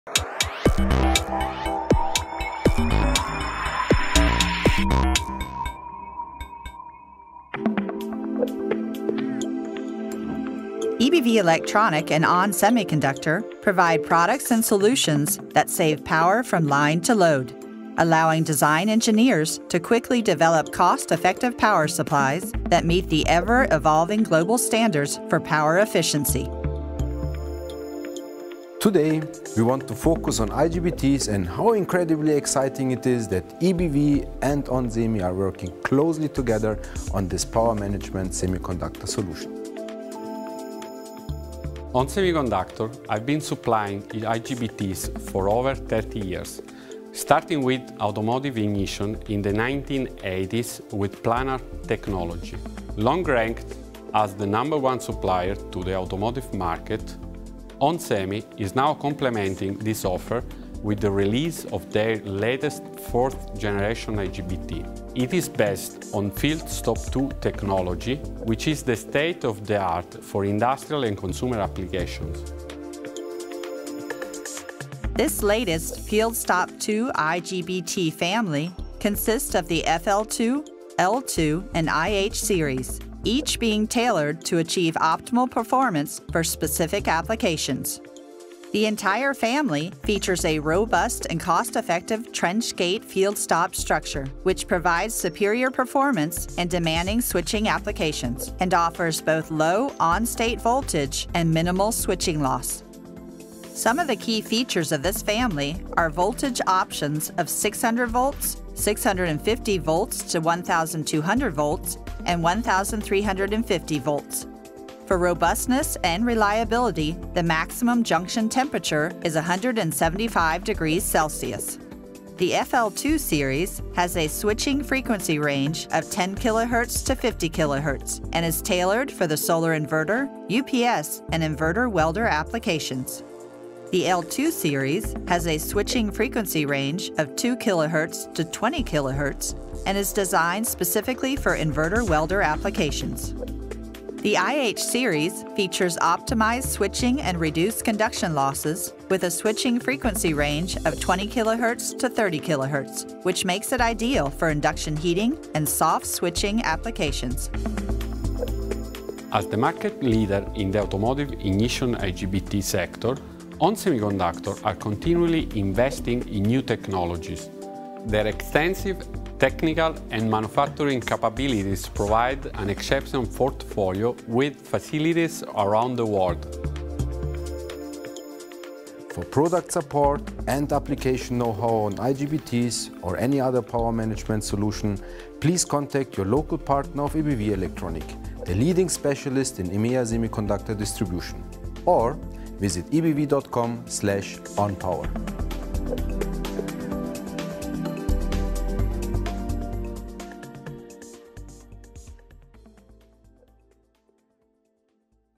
Working together on IGBT - from ON Semi & EBV
ON Semiconductor & EBV Elektronik provide products and solutions that save power from line to load, allowing design engineers to quickly develop cost effective power supplies that meet the ever evolving global standards for power efficiency.